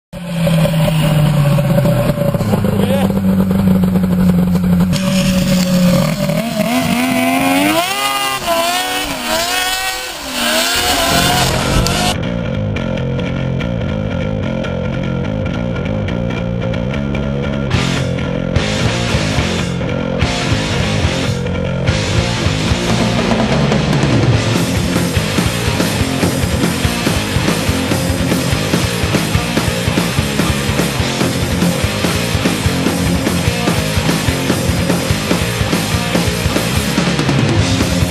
polaris 500 indy classic, swapped XLT 600
my buddy sled. wedge body xlt 600 triple motor, dg can , 136 track